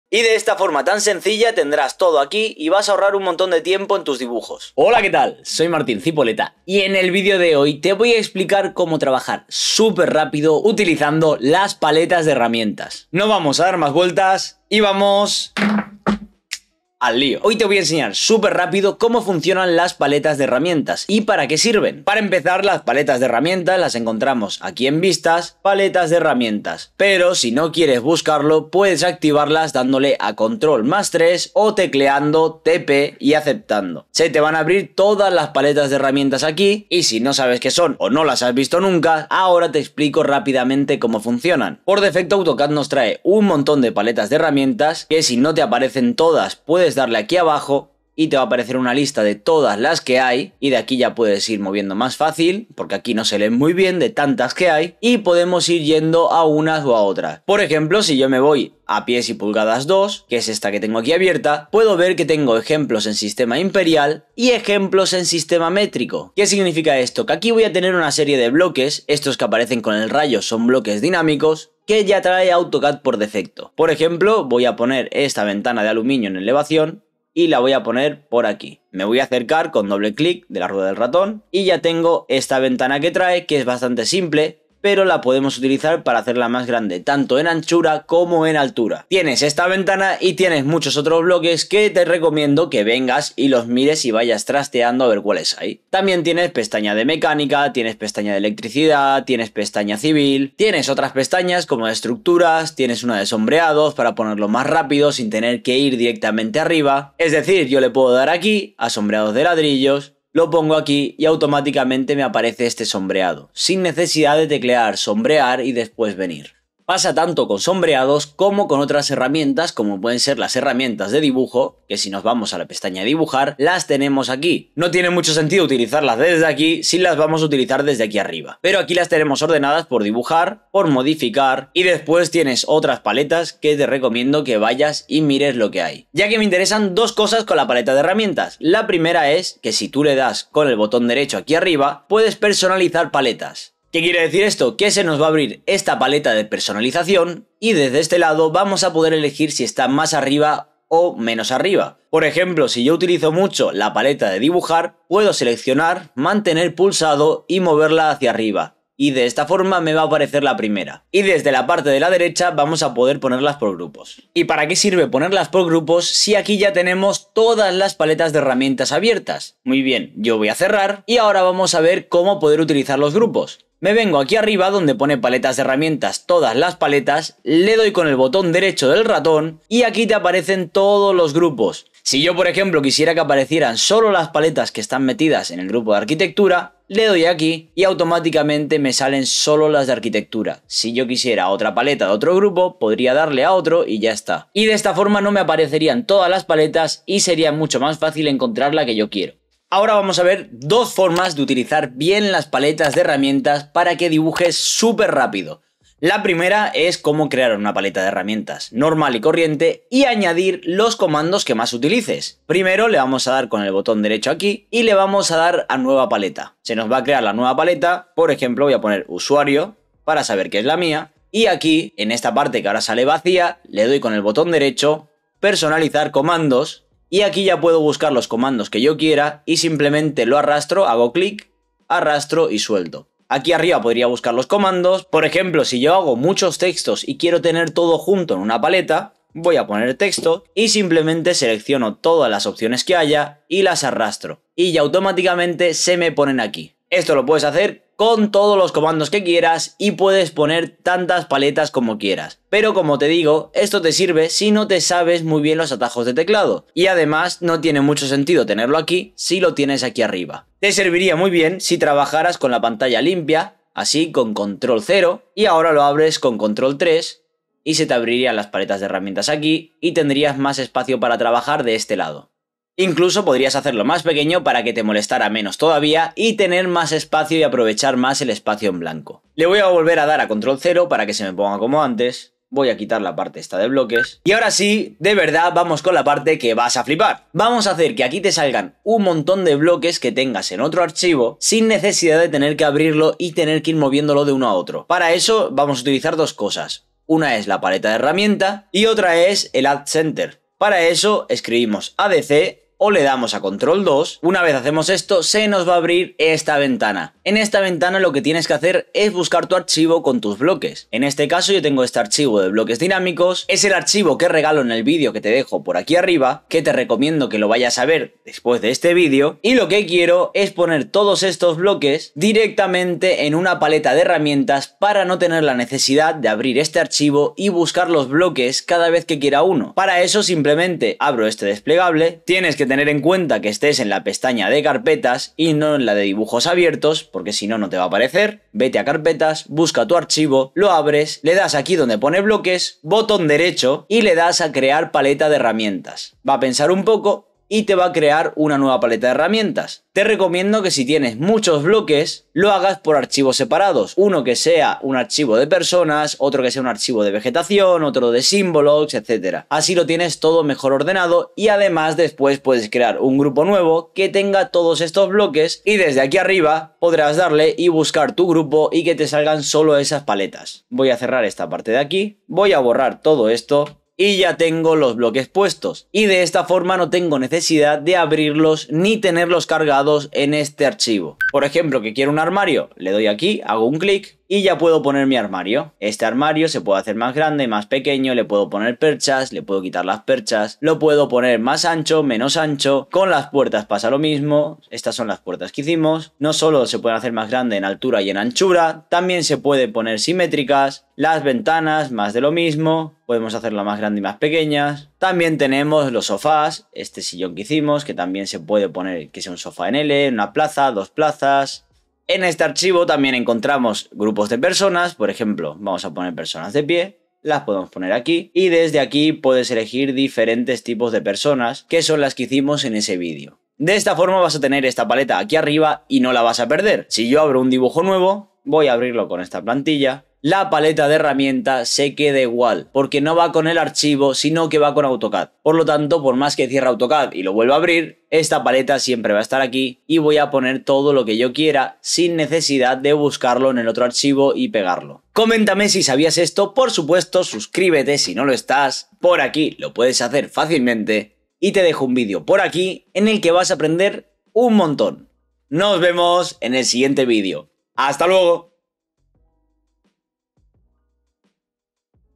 🔴 TRUCO SECRETO de AutoCAD que Debes Saber
Aprende a utilizar las Paletas de Herramientas de AutoCAD para AHORRARTE MUCHO TIEMPO al trabajar.

Utiliza las ToolPalettes de AutoCAD de manera Profesional y consigue crear paletas, ayudándote de otros dibujos, que harán que tu velocidad se multiplique x5 cada vez que quieras utilizar algún bloque.

¿Necesitas clases particulares? 🤨